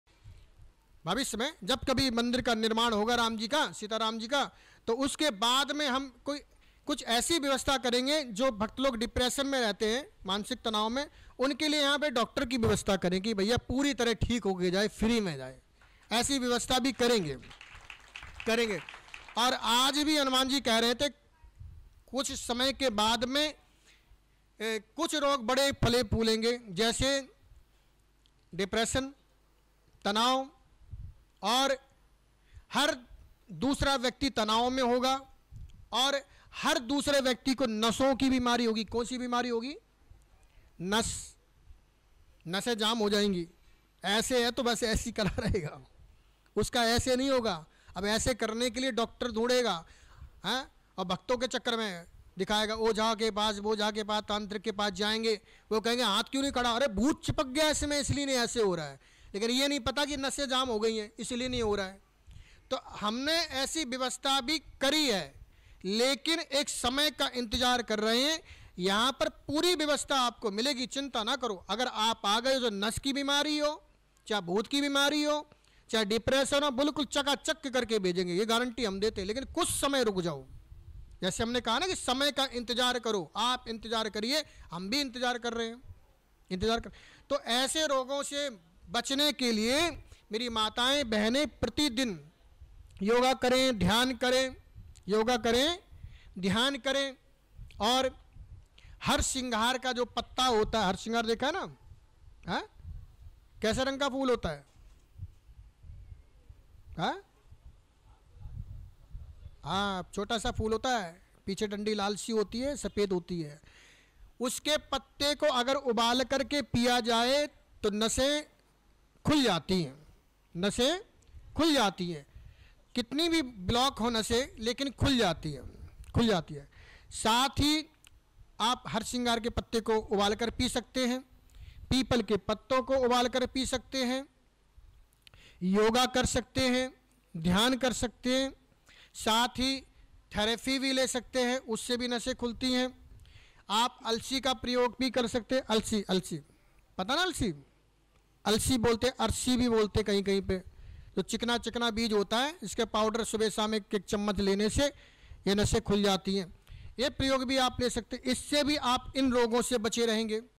तनाव और नसों जैसे बड़ी बीमारी से बचने के लिए करें ये उपाय | विनोद जी महाराज | Sadhna TV
Thanks.